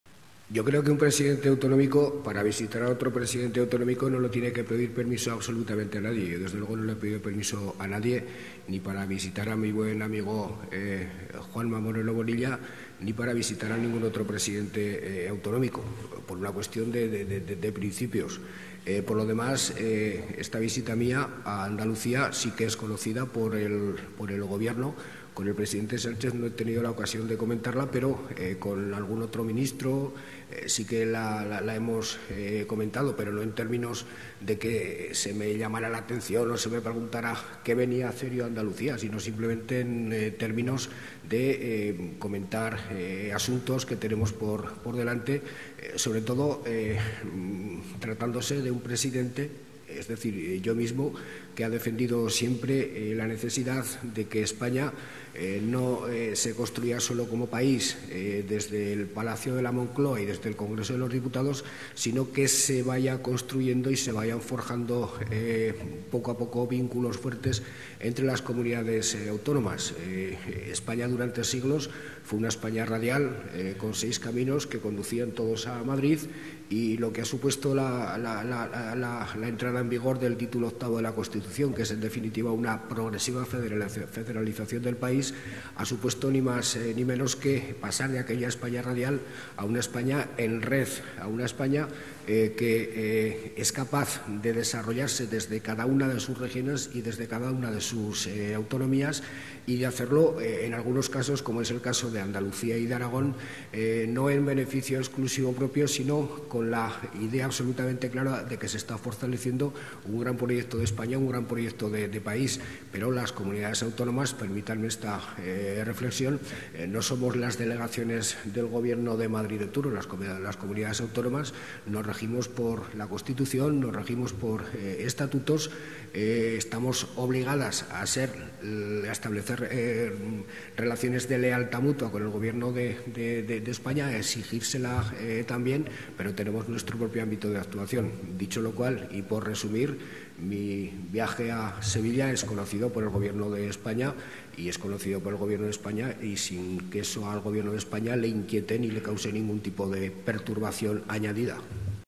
Lambán recuerda que no tiene que pedir permiso "a nadie" para reunirse con Moreno
El presidente del Gobierno de Aragón, Javier Lambán, ha manifestado este jueves que "un presidente autonómico no tiene que pedir permiso a nadie" para reunirse con otro y que no cree que el Ejecutivo central "se inquiete" por el encuentro que ha mantenido este jueves con su homólogo andaluz, Juan Manuel Moreno Bonilla.

(Fuente: Junta de Andalucía)